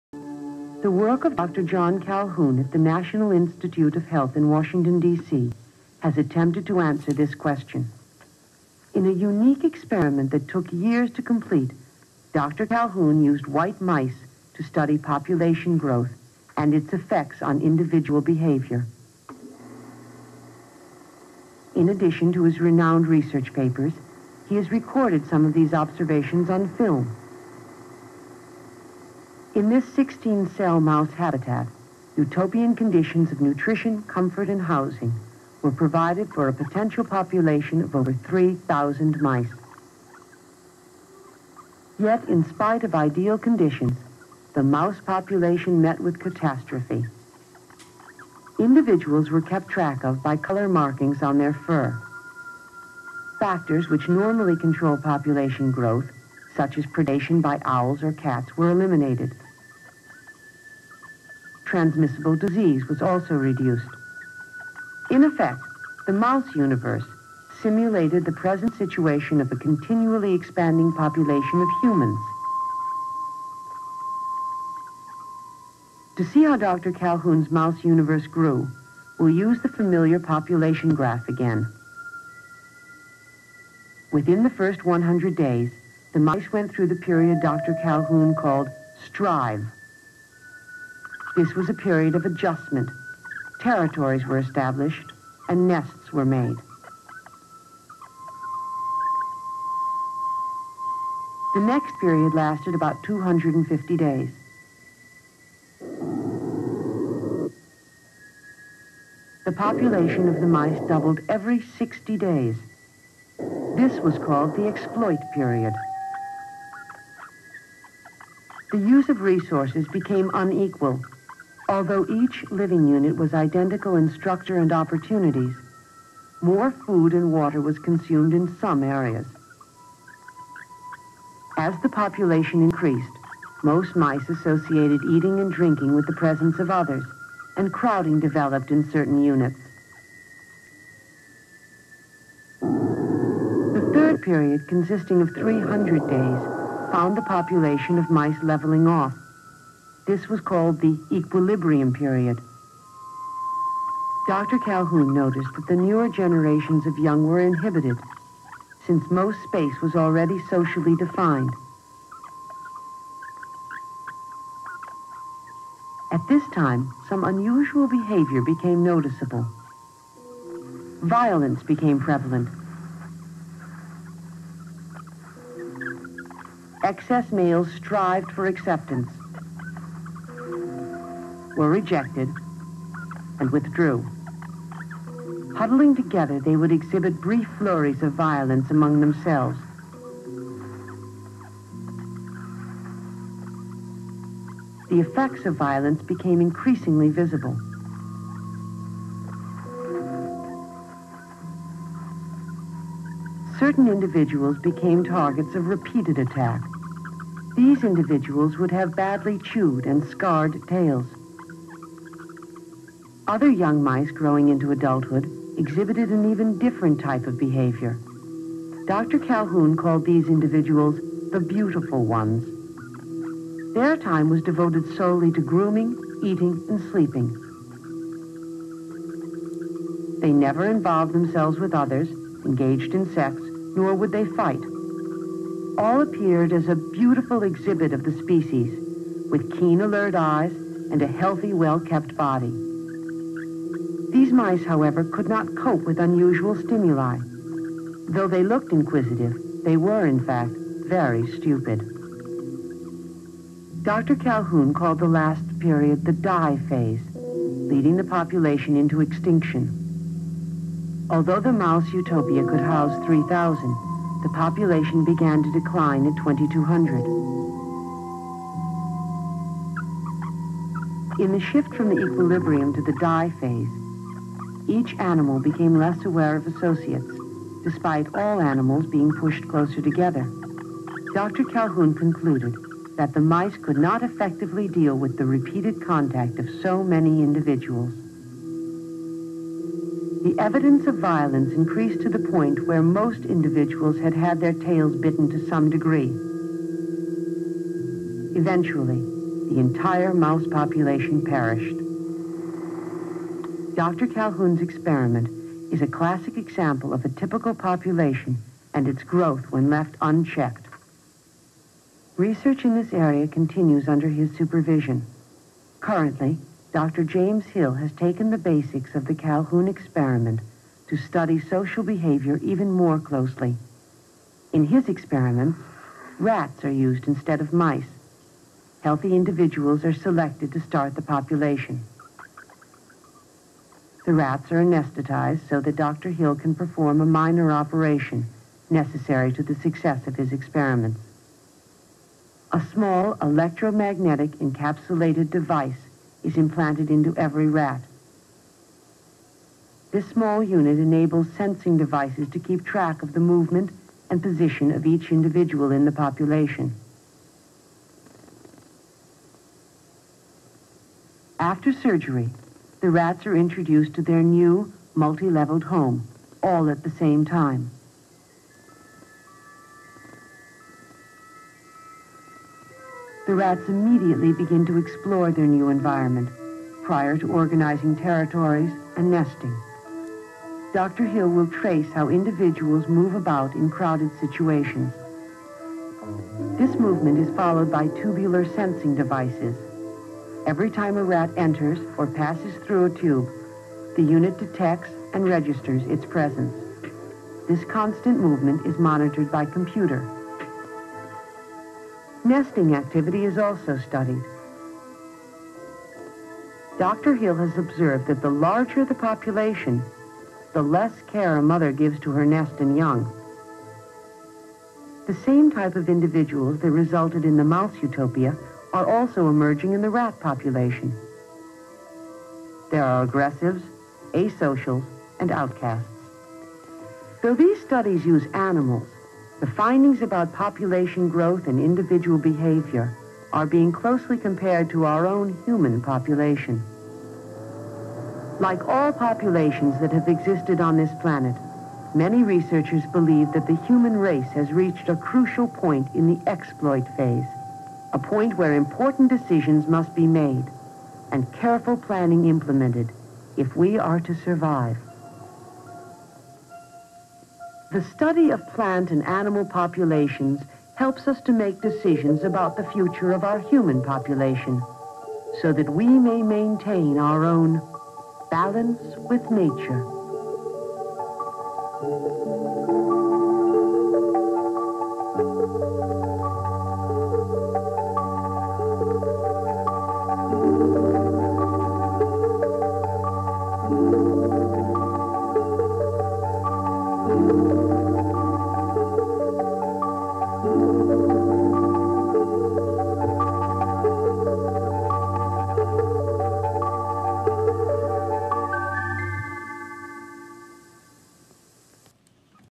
John Calhoun's, ( Mouse Utopia Experiment, 1962. )
These were all part of John Calhoun’s experiments, to study the effects of population density on behavior. But what looked like rat utopias, and mouse paradises at first, quickly spiraled into out-of-control overcrowding, eventual population collapse and seemingly sinister behavior patterns occur.

Excerpt above. (article )

Despite the grim parables presented in Calhoun's observations, he wasn't trying to imply humankind was headed down a similar path toward extinction. While he definitely saw parallels between the downfall of Universe 25 and some of society's ills, he stressed humans — as a more sophisticated species — had the wisdom and ingenuity to reverse such trends.

Excerpt above. (article)

Wikipedia.

Originally posted by.
tamerahunt Channel.

( I only post what You Tube allows me to post, Due to copy right infringement, or Content ID)